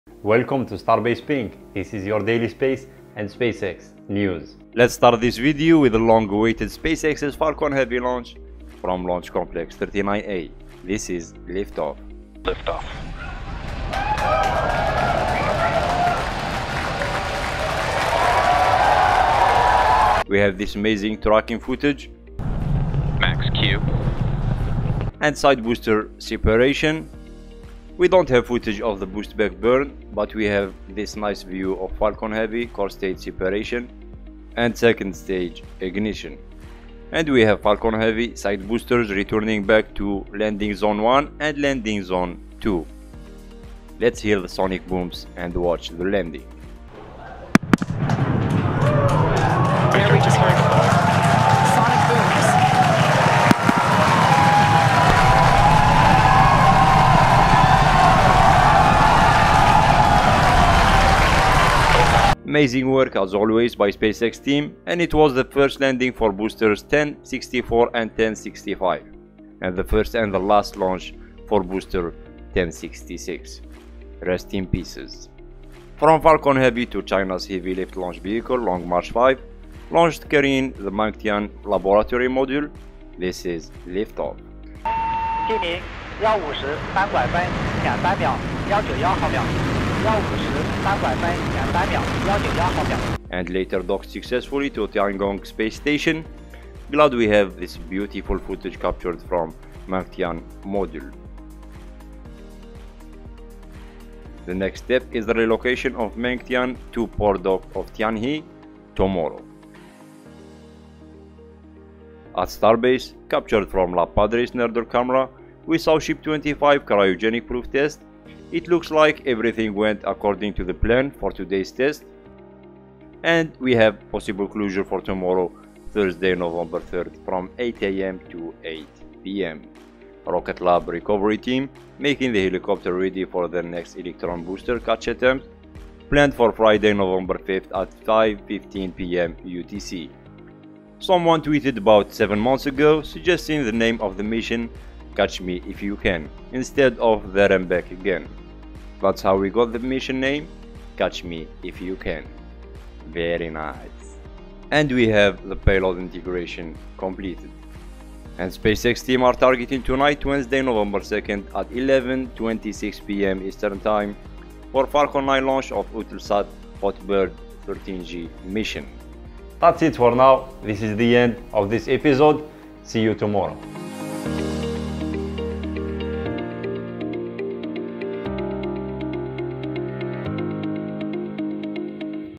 Ship 25 Cryo Proof Test + Mengtian Docking | Starbase Pink
Rocket lab recovery team making the helicopter ready for their next electron booster catch attempt, planned for Friday, Nov 5th at 5:15 pm UTC.

Credit: SpaceX, Sean Cannon ( @planetdeimos ), LabPadre, Cameroncountytx.gov, Stratolaunch, Tory Bruno, CNSA, CSA.

0:00 - Intro
0:04 - Falcon Heavy Liftoff
0:22 - Side Boosters + Stage Separation
0:36 - Boosters 1064 and 1065 Landing
1:15 - Long March 5 Launch
1:33 - Mengtian Docking
1:44 - What's Next?
1:53 - Ship 25 Cryo Proof Test
2:10 - Catch Me If You Can Update
2:38 - Another F9 Launch Tonight
2:49 - Outro